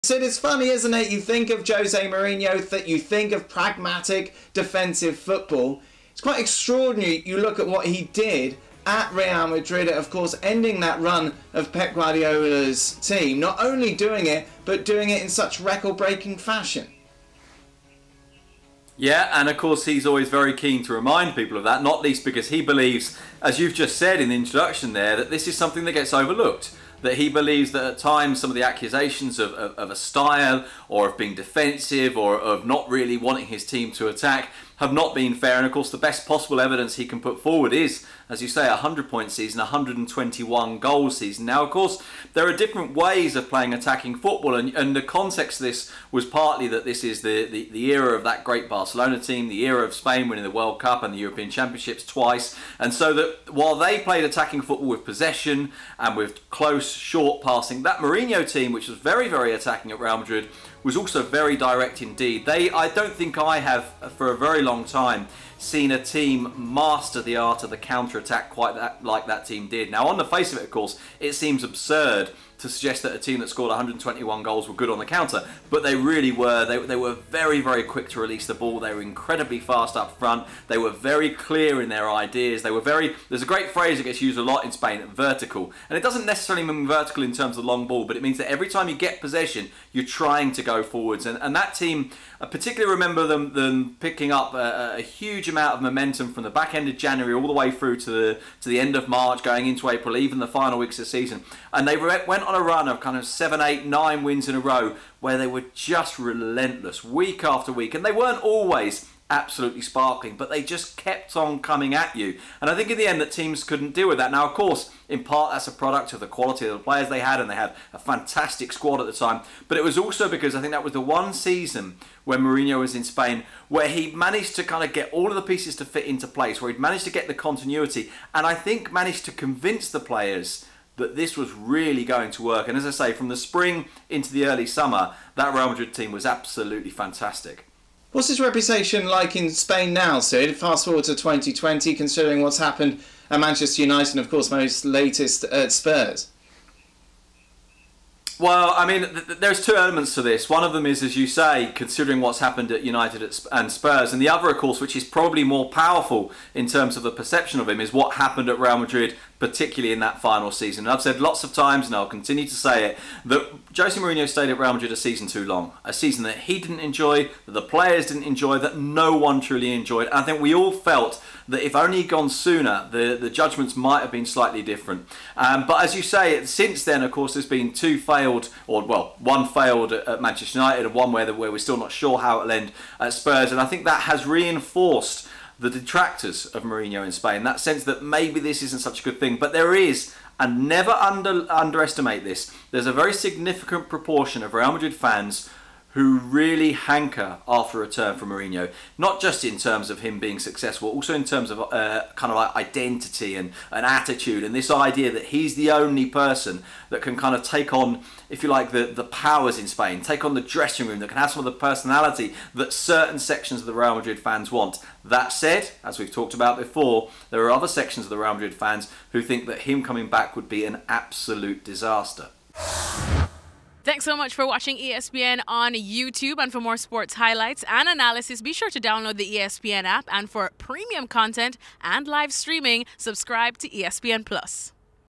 Real Madrid's 2011-12 season revisited: Is a Jose Mourinho return possible? | La Liga
During the 2011-12 season, a Real Madrid team starring Cristiano Ronaldo and managed by Jose Mourinho shattered La Liga records, finishing with 100 points and 121 goals scored. Sid Lowe joins ESPN FC's Dan Thomas to reflect on that season and to discuss how Mourinho still divides Madrid fans.